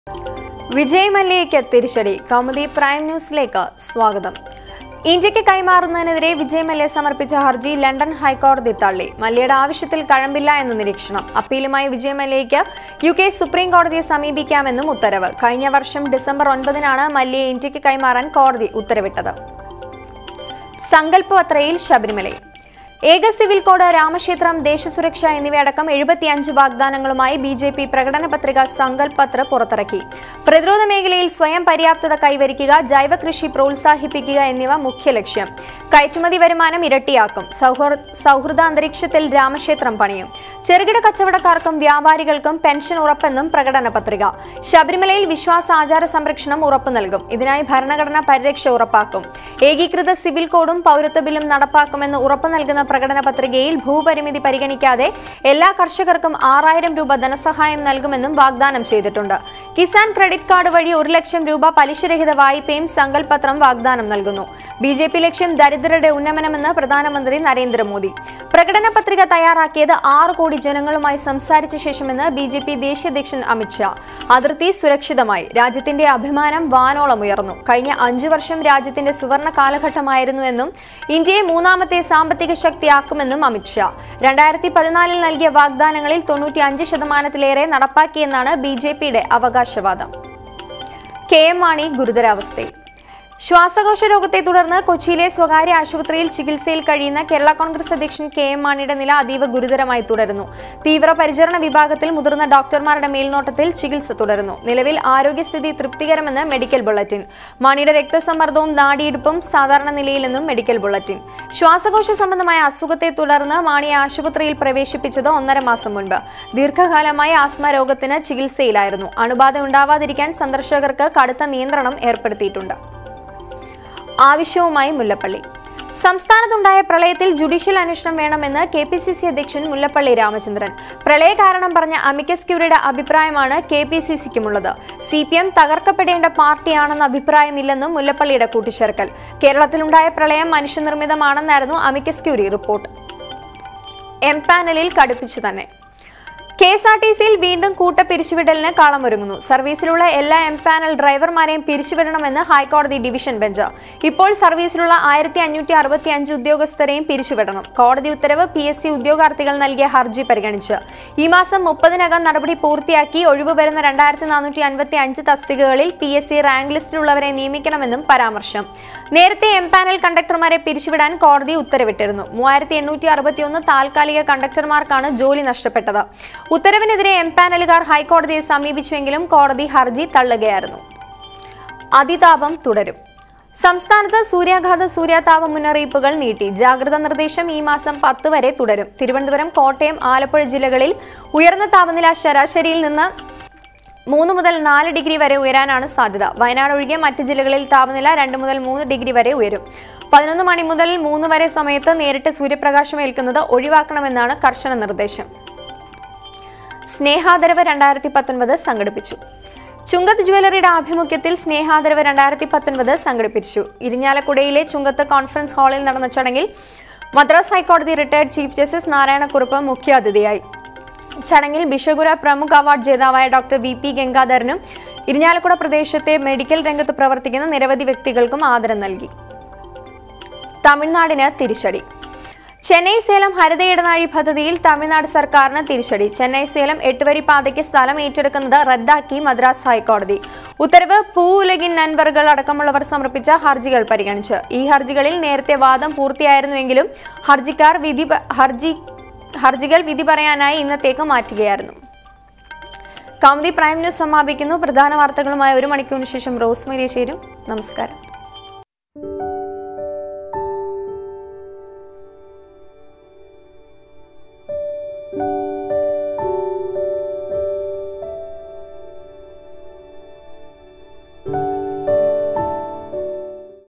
NEWS LIVE | ഇന്ത്യയ്ക്ക് കൈമാറുന്നതിന് എതിരെ വിജയ്മല്യ സമര്‍പ്പി്ച ഹര്‍ജി ലണ്ടന്‍ ഹൈകോടതി തള്ളി
1. ഇന്ത്യയ്ക്ക് കൈമാറുന്നതിന് എതിരെ വിജയ്മല്യ സമര്‍പ്പി്ച ഹര്‍ജി ലണ്ടന്‍ ഹൈകോടതി തള്ളി. മല്യയുടെ ആവശ്യത്തില്‍ കഴമ്പില്ല എന്ന് നിരീക്ഷണം. അപ്പീലുമായി വിജയ് മല്യയ്ക്ക് യു.കെ സുപ്രീംകോടതി സമീപിക്കാം എന്നും ഉത്തരവ്. കഴിഞ്ഞ വര്‍ഷം ഡിസംബര്‍ 9ന് ആണ് മല്യയെ ഇന്ത്യയ്ക്ക് കൈമാറാന്‍ കോടതി ഉത്തരവിട്ടത്
2. ഏക സിവില്‍കോഡ്, രാമക്ഷേത്രം, ദേശസുരക്ഷ എന്നിവ അടക്കം 75 വാഗ്ദാനങ്ങളുമായി ബി.ജെ.പി പ്രകടന പത്രിക സങ്കല്‍പ് പത്ര് പുറത്തിറക്കി. പ്രതിരോധ മേഖലയില്‍ സ്വയം പര്യാപ്തത കൈവരിക്കുക, ജൈവകൃഷി പ്രോത്സാഹിപ്പിക്കുക എന്നിവ മുഖ്യ ലക്ഷ്യം. കയറ്റുമതി വരുമാനം ഇരട്ടി ആക്കും. സൗഹൃദാന്തരീക്ഷത്തില്‍ രാമക്ഷേത്രം പണിയും. ചെറുകിട കച്ചവടക്കാര്‍ക്കും വ്യാപാരികള്‍ക്കും പെന്‍ഷന്‍  ഉറപ്പെന്നും പ്രകടന പത്രിക
3. ശബരിമലയില്‍ വിശ്വാസ, ആചാര സംരക്ഷണം ഉറപ്പു നല്‍കും. ഇതിനായി ഭരണഘടനാ പരിരക്ഷ ഉറപ്പാക്കും. ഏകീകൃത സിവില്‍ കോഡും പൗരത്വബില്ലും നടപ്പാക്കും എന്ന് ഉറപ്പ് നല്‍കുന്ന പ്രകടന പത്രികയില്‍ ഭൂ പരിമധി പരിഗണിക്കാതെ എല്ലാ കര്‍ഷകര്‍ക്കും 6000 രൂപ ധനസഹായം നല്‍കും എന്നും വാഗ്ദാനം ചെയ്തിട്ടുണ്ട്. കിസാന്‍ ക്രെഡിറ്റ് കാര്‍ഡ് വഴി ഒരു ലക്ഷം രൂപ പലിശ രഹിത വായ്പയും സങ്കല്‍പ് പത്രം വാഗ്ദാനം നല്‍കുന്നു. ബി.ജെ.പി ലക്ഷ്യം ദരിദ്രരുടെ ഉന്നമനം എന്ന് പ്രധാനമന്ത്രി നരേന്ദ്രമോദി
4. പ്രകടന പത്രിക തയ്യാറാക്കിയത്, ആറ് കോടി ജനങ്ങളുമായി സംസാരിച്ച ശേഷം എന്ന് ബി.ജെ.പി ദേശീയ അധ്യക്ഷന്‍ അമിത് ഷാ. അതിര്‍ത്തി സുരക്ഷിതമായി. രാജ്യത്തിന്റെ അഭിമാനം വാനോളം ഉയര്‍ന്നു. കഴിഞ്ഞ അഞ്ചു വര്‍ഷം രാജ്യത്തിന്റെ സുവര്‍ണ കാലഘട്ടം ആയിരുന്നു എന്നും. ഇന്ത്യയെ മൂന്നാമത്തെ സാമ്പത്തിക ശക്തി ആക്കും എന്നും അമിത് ഷാ. 2014-ല്‍ നല്‍കിയ വാഗ്ദാനങ്ങളില്‍ 95 ശതമാനത്തിലേറെ നടപ്പാക്കി എന്നാണ് ബി.ജെ.പിയുടെ അവകാശവാദം.
5. ശ്വാസകോശ രോഗത്തെ തുടര്‍ന്ന് കൊച്ചിയിലെ സ്വകാര്യ ആശുപത്രിയില്‍ ചികിത്സയില്‍ കഴിയുന്ന കേരള കോണ്‍ഗ്രസ് അധ്യക്ഷന്‍ കെ.എം. മാണിയുടെ നില അതീവ ഗുരുതരമായി തുടരുന്നു. തീവ്രപരിചരണ വിഭാഗത്തില്‍ മുതിര്‍ന്ന ഡോക്ടര്‍മാരുടെ  മേല്‍നോട്ടത്തില്‍ ചികിത്സ തുടരുന്നു. നിലവില്‍ ആരോഗ്യസ്ഥിതി തൃപ്തികരം എന്ന് ബെഡിക്കല്‍ ബുള്ളറ്റിന്‍. മാണിയുടെ രക്ത സമ്മര്‍ദ്ദവും നാഡിമിടിപ്പും സാധാരണ നിലയില്‍ എന്നും മെഡിക്കല്‍ ബുള്ളറ്റിന്‍
6. ശ്വാസകോശ സംബന്ധമായ അസുഖത്തെ തുടര്‍ന്ന് മാണിയെ ആശുപത്രിയില്‍ പ്രവേശിപ്പിച്ചത് ഒന്നരമാസം മുന്‍പ്. ദീര്‍ഘകാലമായി ആസ്മ രോഗത്തിന് ചികിത്സയില്‍ ആയിരുന്നു. അണുബാധ ഉണ്ടാവാതിരിക്കാന്‍ സന്ദര്‍ശകര്‍ക്ക് കടുത്ത നിയന്ത്രണം ഏര്‍പ്പെടുത്തിയിട്ടുണ്ട്
7. സംസ്ഥാനത്ത് ഉണ്ടായ പ്രളയത്തില്‍ ജുഡീഷ്യല്‍ അന്വേഷണം വേണമെന്ന് കെ.പി.സി.സി അധ്യക്ഷന്‍ മുല്ലപ്പള്ളി രാമചന്ദ്രന്‍. പ്രളയ കാരണം പറഞ്ഞ അമികസ്‌ക്യൂരിയുടെ അഭിപ്രായം ആണ് കെ.പി.സി.സിയ്ക്കും ഉള്ളത്. സി.പി.എം തകര്‍ക്കപ്പെടേണ്ട പാര്‍ട്ടി ആണ് എന്ന അഭിപ്രായം ഇല്ലെന്നും മുല്ലപ്പള്ളിയുടെ കൂട്ടിച്ചേര്‍ക്കല്‍. കേരളത്തില്‍ ഉണ്ടായ പ്രളയം മനുഷ്യ നിര്‍മ്മിതം ആണ് എന്നായിരുന്നു അമികസ്‌ക്യൂരി റിപ്പോര്‍ട്ട്
8. കെ.എസ്.ആര്‍.ടി.സിയില്‍ വീണ്ടും കൂട്ടപ്പിരിച്ചു വിടലിന് കളം ഒരുങ്ങുന്നു. സര്‍വീസിലുള്ള എല്ലാ  എം പാനല്‍ ഡ്രൈവര്‍മാരേയും പിരിച്ചു വിടണം എന്ന് ഹൈക്കോടതി ഡിവിഷന്‍ ബെഞ്ച്. ഇപ്പോള്‍ സര്‍വീസിലുള്ള 1565 ഉദ്യോഗസ്ഥരേയും പിരിച്ചു വിടണം. കോടതി ഉത്തരവ്, പി.എസ്.സി ഉദ്യോഗാര്‍ത്ഥികള്‍ നല്‍കിയ ഹര്‍ജി പരിഗണിച്ച്. ഈ മാസം 30ന് അകം നടപടി പൂര്‍ത്തി ആക്കി ഒഴിവുവരുന്ന 2455 തസ്തികകളില്‍ പി.എസ്.സി റാങ്ക്ലിസ്റ്റില്‍ ഉള്ളവരെ നിയമിക്കണം എന്നും പരാമര്‍ശം
9. നേരത്തെ എം പാനല്‍ കണ്ടക്ടര്‍മാരെ പിരിച്ചു വിടാന്‍ കോടതി നേരത്തെ ഉത്തരവിട്ടിരുന്നു. 3861 താത്കാലിക കണ്ടക്ടര്‍മാര്‍ക്കാണ് ജോലി നഷ്ടപ്പെട്ടത്. ഉത്തരവിന് എതിരെ എം പാനലുകാര്‍ ഹൈക്കോടതിയെ സമീപിച്ചു എങ്കിലും കോടതി ഹര്‍ജി തള്ളുക ആയിരുന്നു
10.  സംസ്ഥാനത്ത് സൂര്യാഘാത, സൂര്യതാപ മുന്നറിയിപ്പുകള്‍ നീട്ടി. ജാഗ്രതാ നിര്‍ദ്ദേശം ഈ മാസം 10വരെ തുടരും. തിരുവനന്തപുരം, കോട്ടയം, ആലപ്പുഴ ജില്ലകളില്‍ ഉയര്‍ന്ന താപനില ശരാശരിയില്‍ നിന്ന് മൂന്ന് മുതല്‍ നാല് ഡിഗ്രി വരെ ഉയരാന്‍ ആണ് സാധ്യത. വയനാട് ഒഴികെ മറ്റ് ജില്ലകളില്‍ താപനില 2 മുതല്‍ 3 ഡിഗ്രി വരെ ഉയരും. 11 മണി മുതല്‍ 3 വരെ സമയത്ത് നേരിട്ട് സൂര്യപ്രകാശം ഏല്‍ക്കുന്നത് ഒഴിവാക്കണം എന്നാണ് കര്‍ശന നിര്‍ദ്ദേശം.
11. ചെന്നൈ-സേലം ഹരിത ഇടനാഴി പദ്ധതിയില്‍ തമിഴ്നാട് സര്‍ക്കാരിന് തിരിച്ചടി. ചെന്നൈ- സേലം എട്ടുവരി പാതയ്ക്ക് സ്ഥലം ഏറ്റെടുക്കുന്നത് റദ്ദാക്കി മദ്രാസ് ഹൈക്കോടതി. ഉത്തരവ്, പൂ ഉലകിന്‍ നന്‍പര്‍കള്‍ അടക്കമുള്ളവര്‍ സമര്‍പ്പിച്ച ഹര്‍ജികള്‍ പരിഗണിച്ച്. ഈ ഹര്‍ജികളില്‍ നേരത്തെ വാദം പൂര്‍ത്തി ആയിരുന്നു എങ്കിലും ഹര്‍ജികള്‍ വിധി പറയാനായി ഇന്നത്തേക്ക് മാറ്റുക ആയിരുന്നു